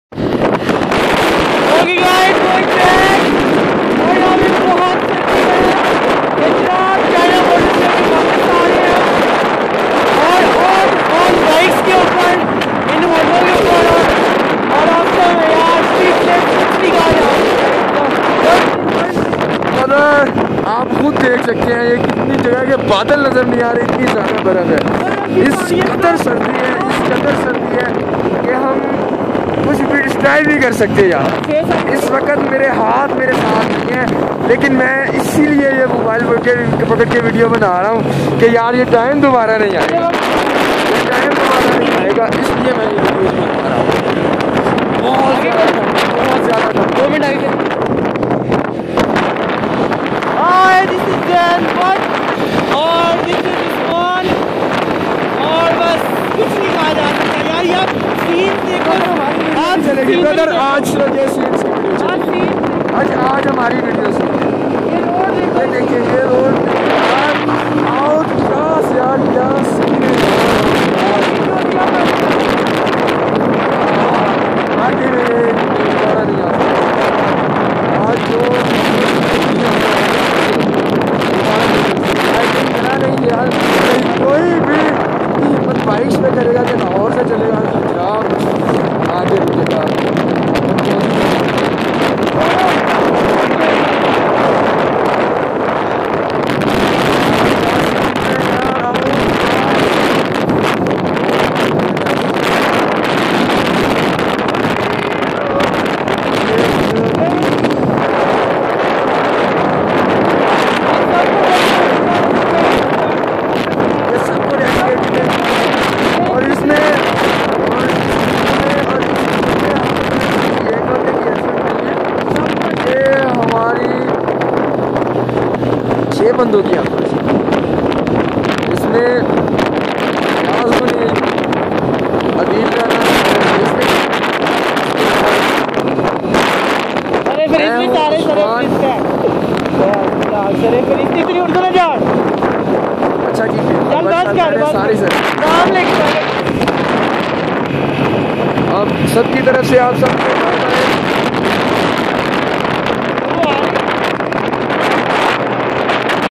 Ride on Bikes Lahore too Khunjerab Pass (Pak-china Border)
Trip lahore too khunjerab pass on Bikes amazing view Must watch thiss